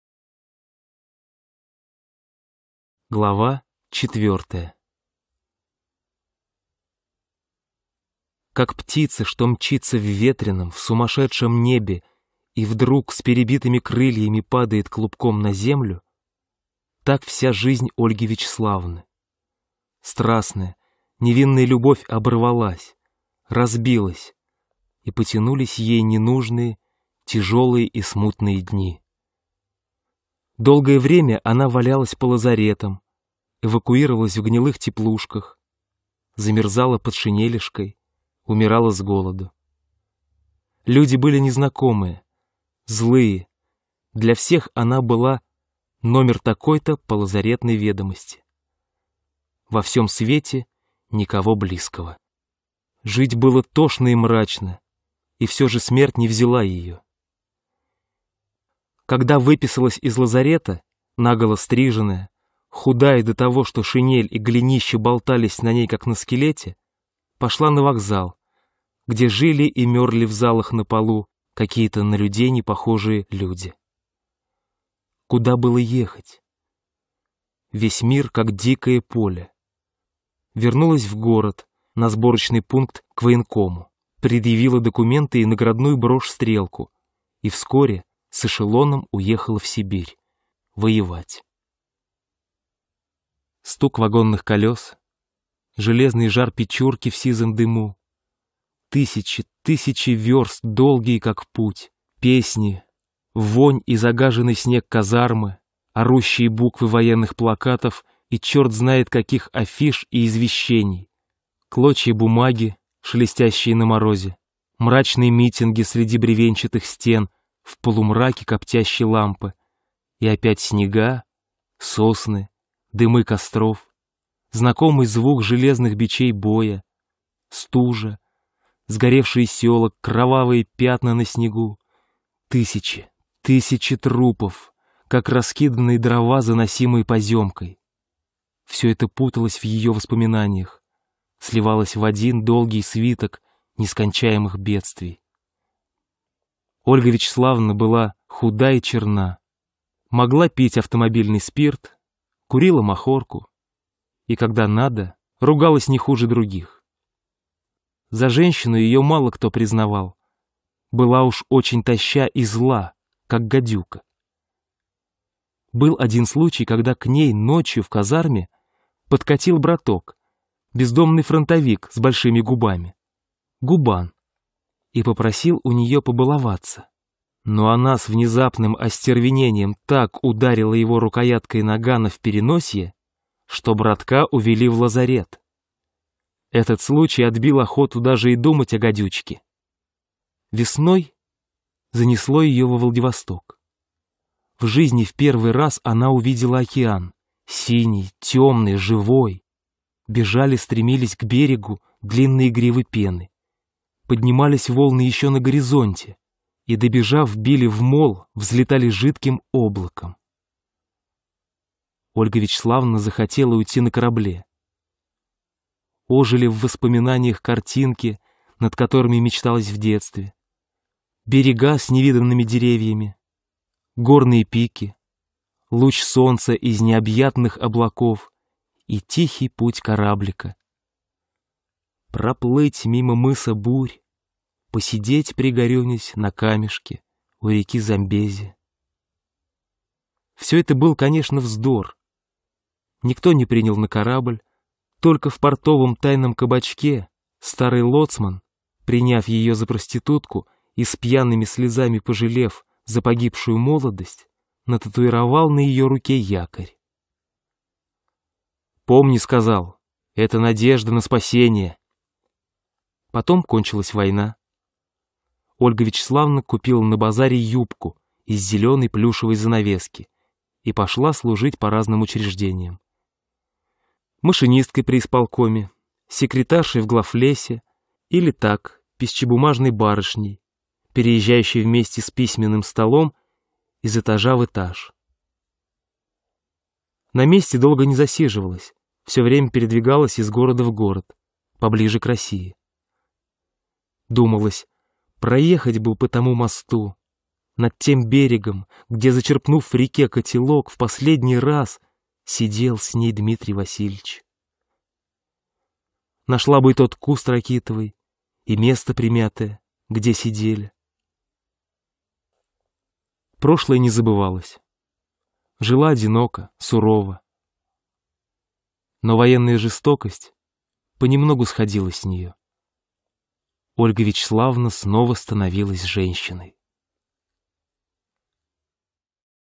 2000152 Glava 04 Аудиокнига. Толстой Алексей Николаевич. "Гадюка"
Описание
Граф Алексей Николаевич Толстой (1882  — 1945) — русский советский писатель и общественный деятель из рода Толстых. Автор социально-психологических, исторических и научно-фантастических романов, повестей и рассказов, публицистических произведений.
Героиня повести «Гадюка» Ольга Вячеславовна Зотова принадлежала к буржуазной зажиточной семье, и жизнь планировалась соответствующе: «муж -- приличный блондин, я -- в розовом пеньюаре, сидим, оба отражаемся в никелированном кофейнике. И больше -- ничего!.. И это -- счастье...» Однако гражданская война внесла свои коррективы. Бандиты убили родителей, зверски изувечили ее саму, разворовали и сожгли дом... Потеряв то, что составляло основу и смысл существования, Зотова встала перед выбором новой своей роли во враждебном и страшном мире. Она стала другим человеком с новым классовым сознанием. После гражданской войны Ольга Вячеславовна попадает в другой мир, служит писчебумажной барышней по разным учреждениям. Но к этой третьей жизни она так и не смогла привыкнуть...

Аудиокнига.